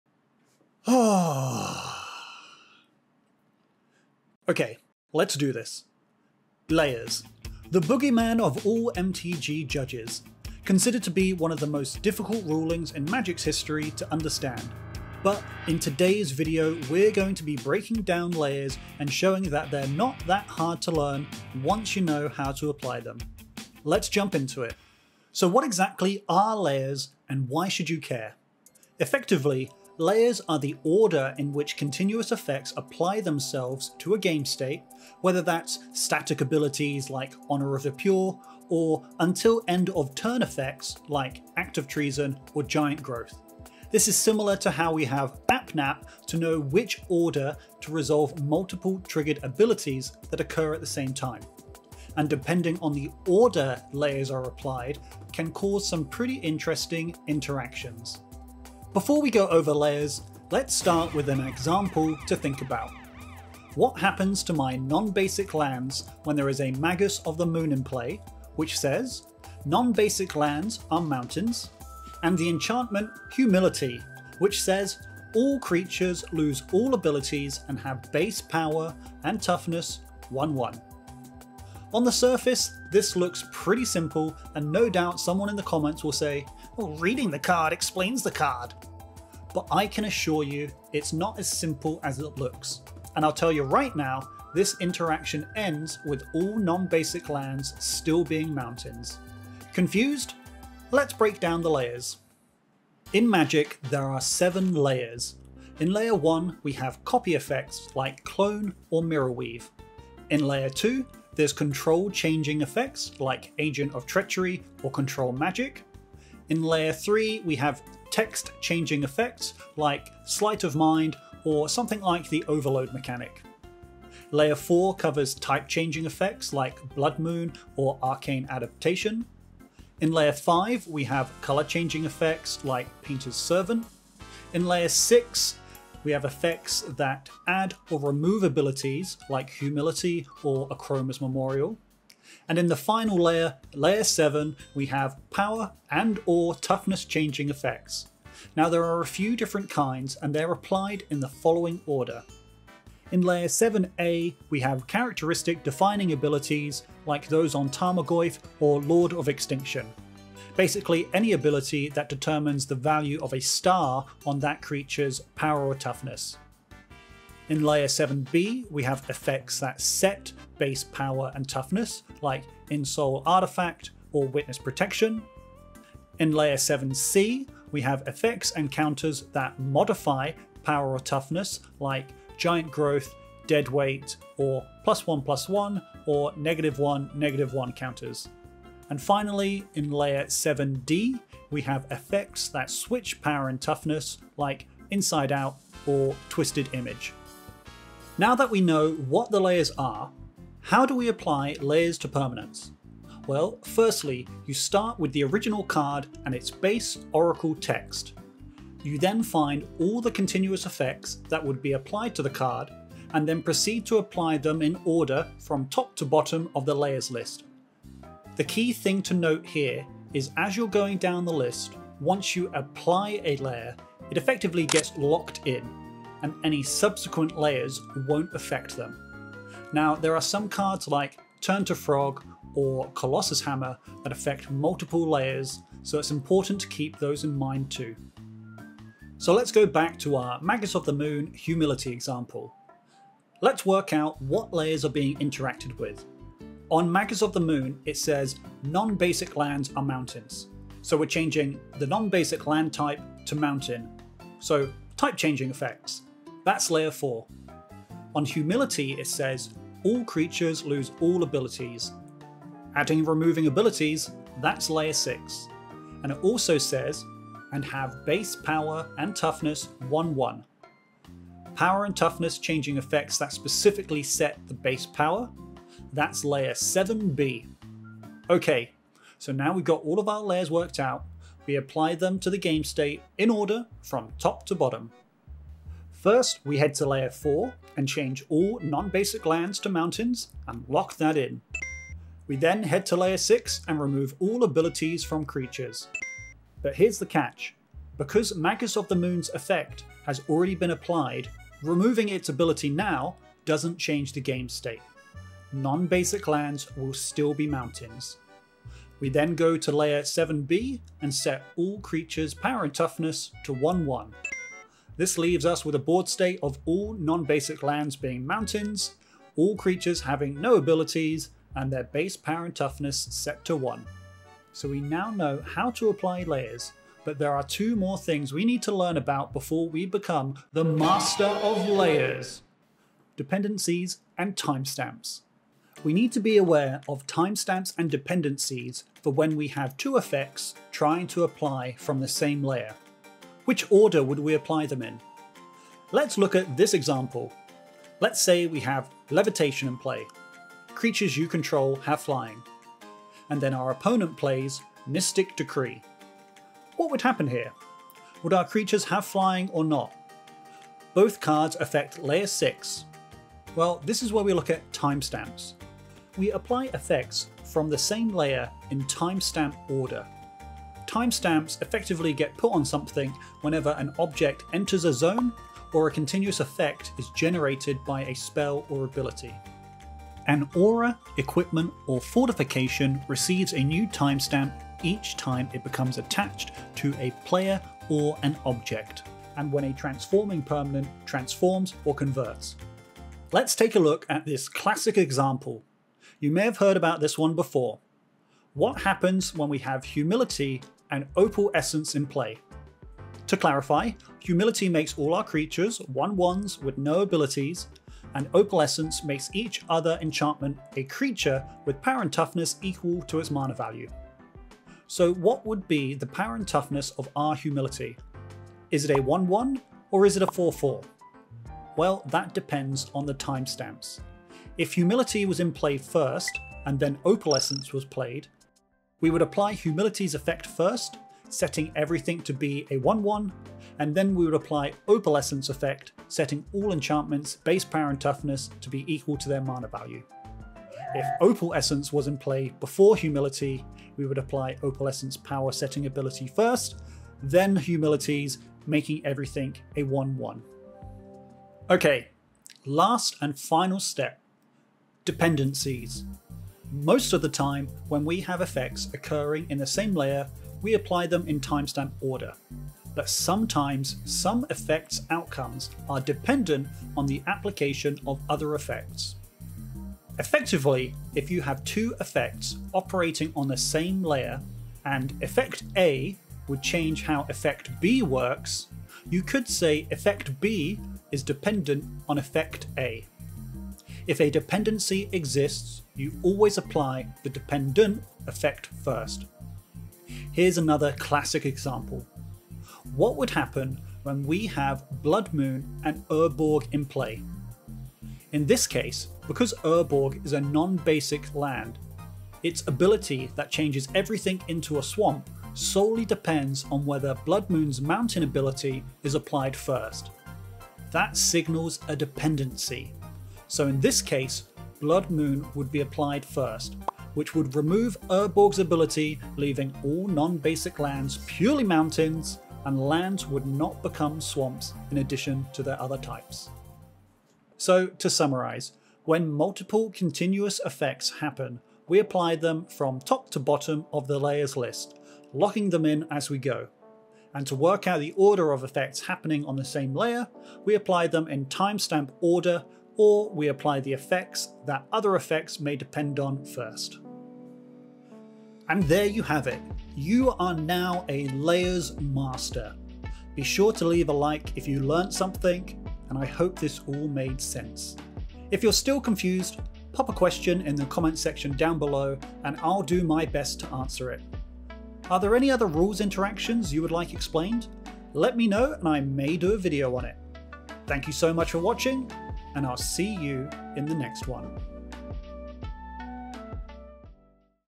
MTGs HARDEST rule is actually EASY - Let me explain: Layers for Beginners
It's finally here, the Layers video. Layers is notorious for being MTGs hardest rule to wrap your head around. In this video, I break down and explain Layers, Timestamps, Dependencies and cover everything you'll need to know about Layers, so you can apply them in you next game of Commander!

0:00 Intro
0:24 What are Layers?
1:00 Layers practice example
1:42 The 7 Layers
3:17 How to apply Layers
3:59 Answering the example
5:32 Timestamps
6:10 When do Timestamps get applied?
6:33 Timestamp example
7:31 Dependencies
8:10 Dependencies example
8:46 Layers Summary